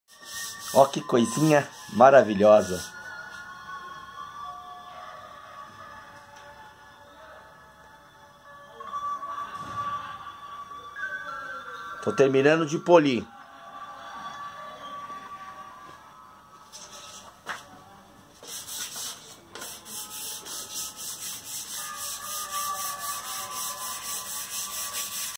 Katana terminando de ser polida coisinha maravilhosa !
Tachi forjado
Sukemasa Shojo
Aço carbono T10
hamon suguha
Tamanho lâmina com 78 cm
cabo 28 cm
Nakago 21 cm
Assinada
Peso 1180 gramas
Largura 2.8 cm habaki
2 cm yokote

espessura 7 mm habaki
5,2 mm no Yokote

montagem koshirae handachi para batalha com saya de metal

Curvatura , Saki Zori

site